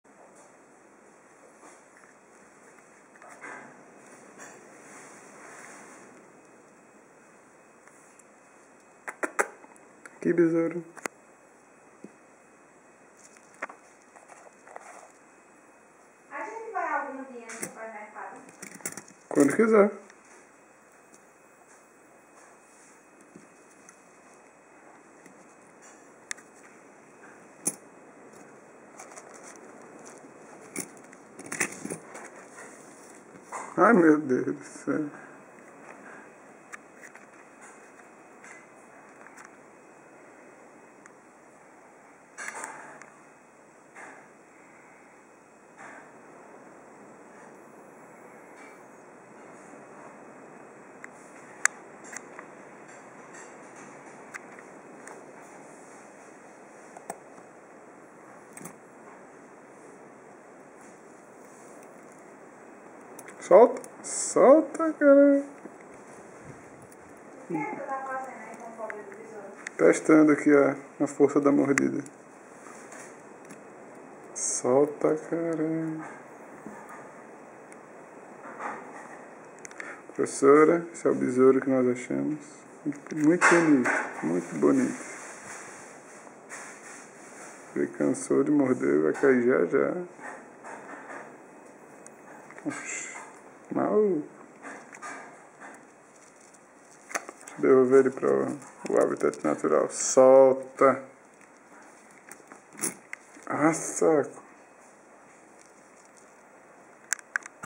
Besouro (Carabidae) com forte mandíbula.
Besouro encontrado em Patos-PB. Deve doer a mordidinha dele viu =p...